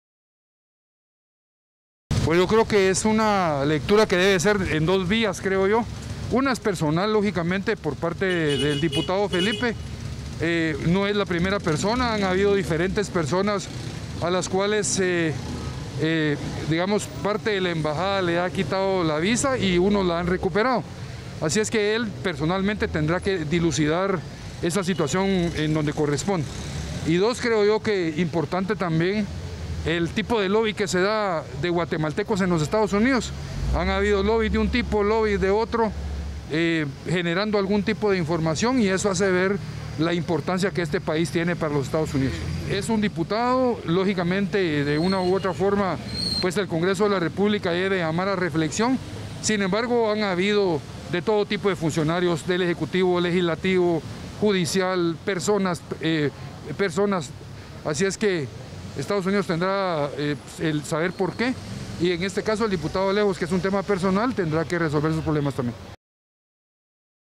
Felipe Alejos deberá aclarar su situación
Tras el anuncio del gobierno de los Estados Unidos de cancelar las visas al congresista de la bancada Todos Felipe Alejos y su familia, el diputado Boris España dijo que este es un problema personal y que no es el primer caso que ocurre.